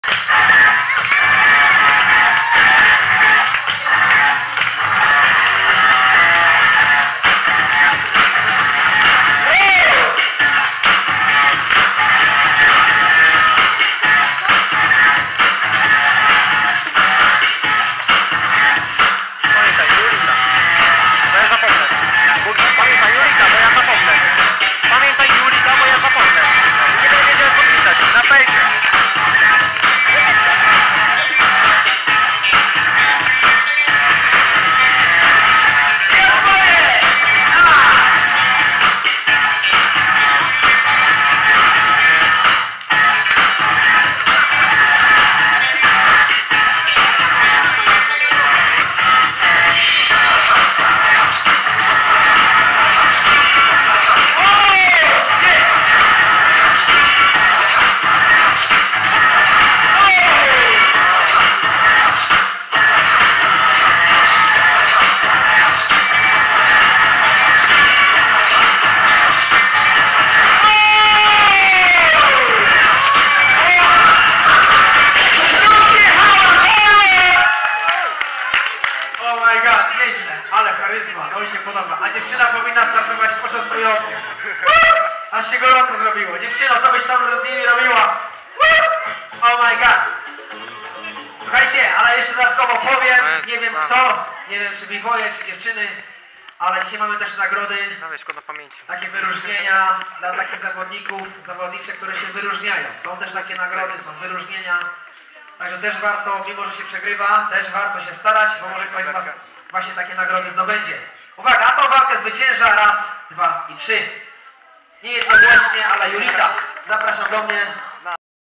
??? vs Julita | Andrzejkowy Turniej Breakdance 2015
Bitwa w kategorii bgirls powyżej lat 12.

Winner: Julita